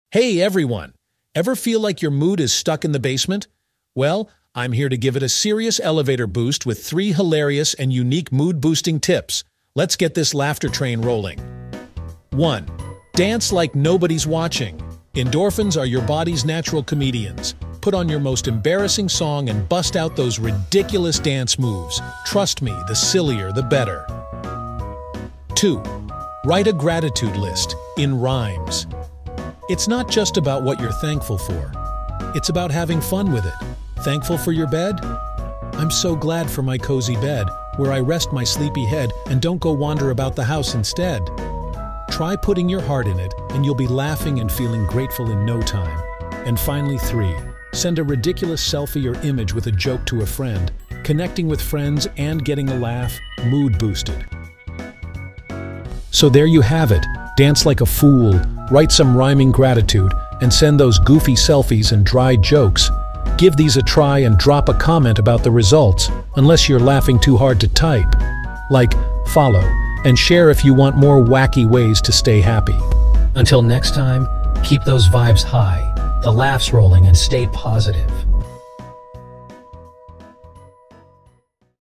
3 Silly and Fun Ways to Instantly Boost Your Mood | Quick Tips for a Happier Day
Feeling a little down or in need of a quick mood boost? In this video, we'll share 3 silly yet effective ways to instantly lift your spirits and put a smile on your face! From playful activities to lighthearted tricks, these tips are easy to try and perfect for any time you need a quick pick-me-up. If you're ready for a dose of happiness and a burst of laughter, this video is for you.